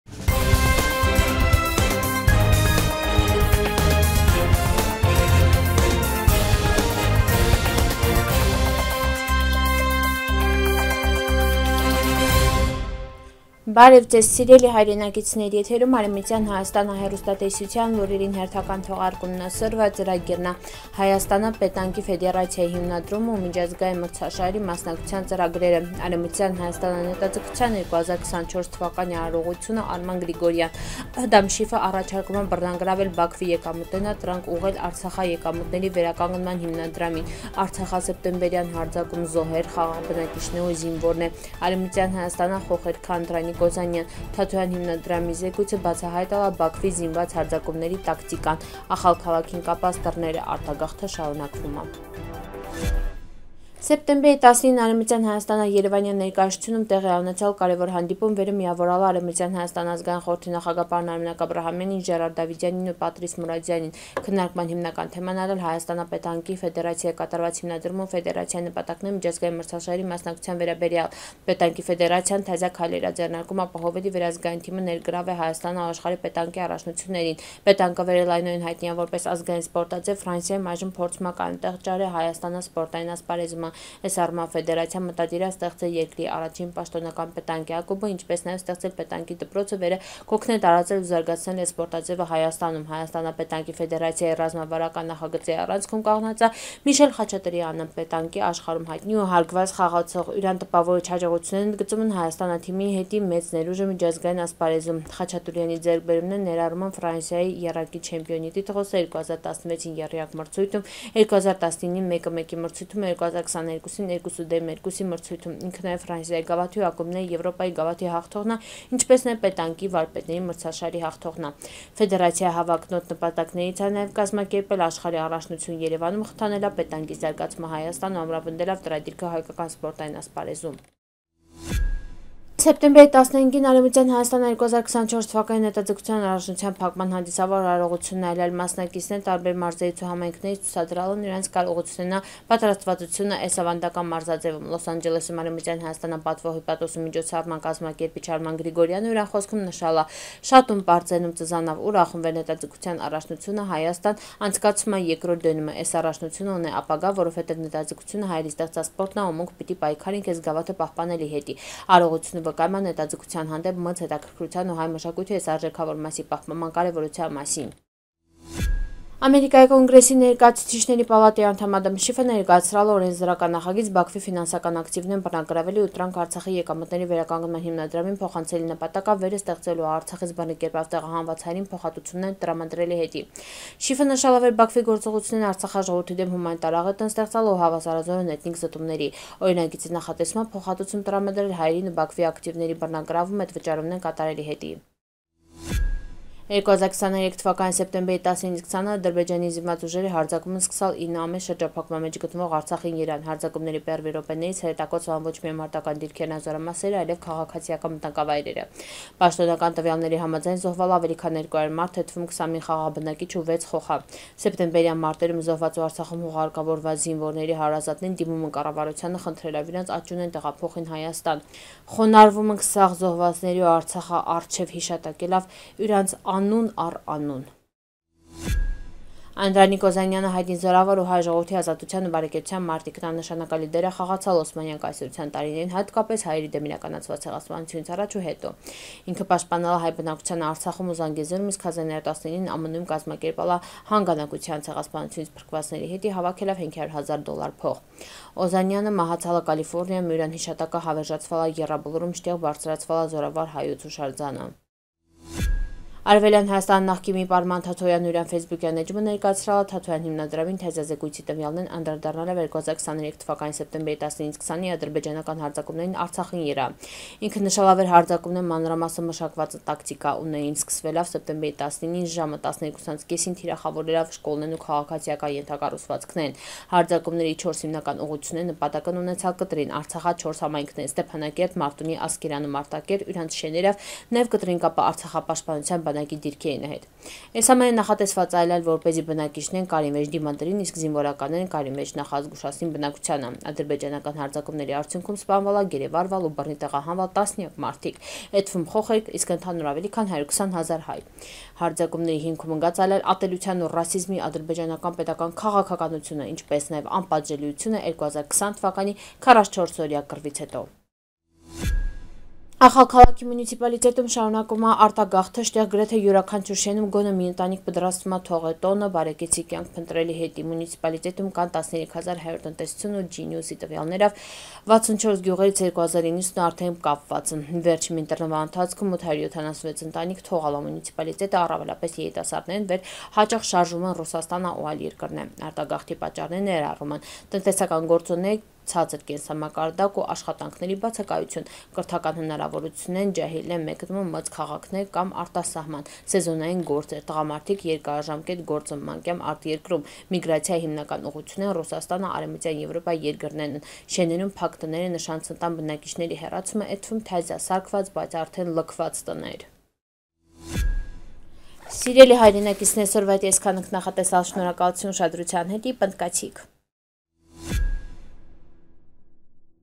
Արցախա սեպտեմբերյան հարձակում․ զոհեր, խաղաղ բնակիչներ ու զինվորներ
1. Հայաստանա պետանկի ֆեդերացիայի հիմնադրումը ու միջազգային մրցաշարերի մասնակցության ծրագրերը
2. Արեւմտեան Հայաստանա նետաձգության 2024 արարողությունը.  Արման Գրիգորյան -
3. Ադամ Շիֆն առաջարկումա բռնագրավել Բաքվի եկամուտներն ու տրանք ուղղել «Արցախա եկամուտների վերականգնման հիմնադրամին» -
4. Արցախա սեպտեմբերյան հարձակում․ զոհեր, խաղաղ բնակիչներ ու զինվորներ
5. Արեւմտեան Հայաստանա խոխերքը. Անդրանիկ Օզանյան
6. Թաթոյան հիմնադրամի զեկույցը բացահայտալա Բաքվի զինված հարձակումների տակտիկան
7. Ախալքալաքին կապած  տռները. արտագաղթը շարունակվումա